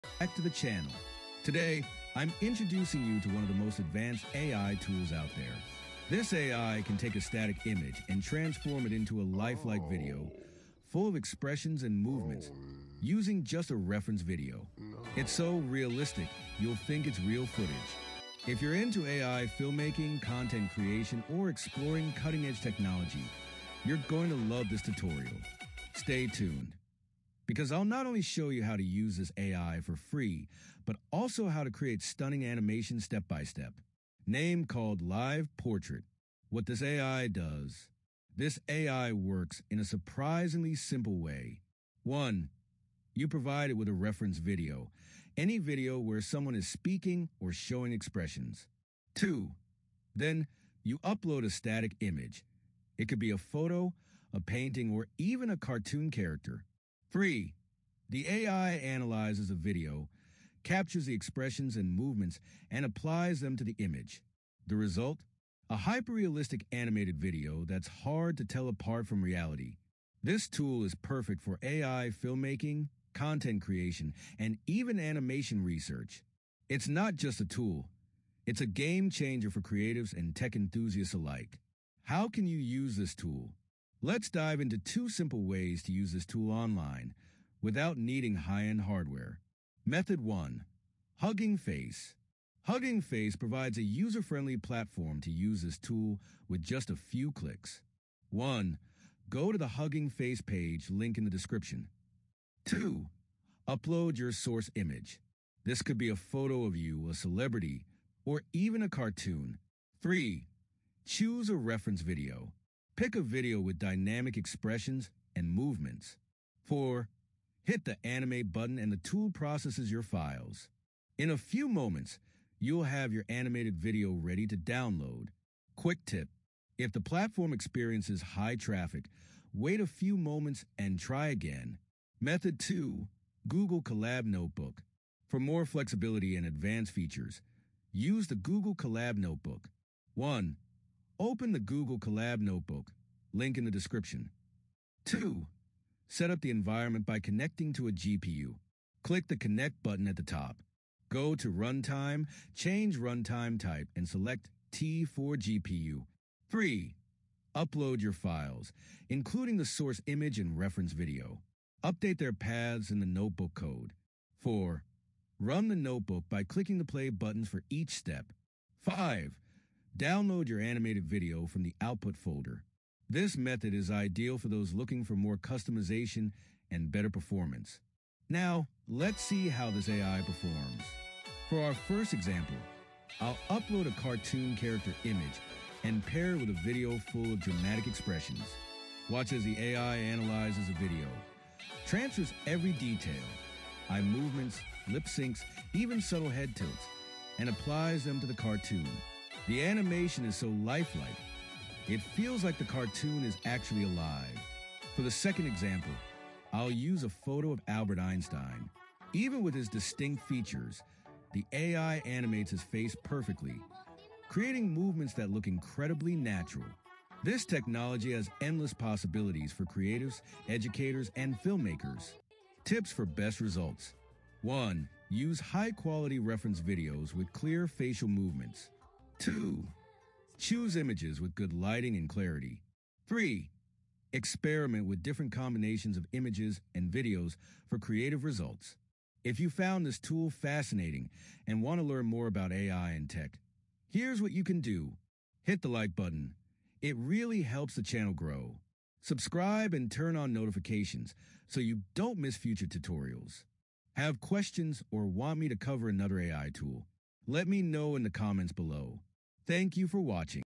CREATE MIND-BLOWING Image Animations with LivePortrait!
This FREE Image to Video Ai Can Control Expressions and Emotion | Live Portrait Google Colab

In this video I will share ai video generator that can animate your still images with realistic expressions and emotions. This is called live portrait. This free image to video ai is game changer.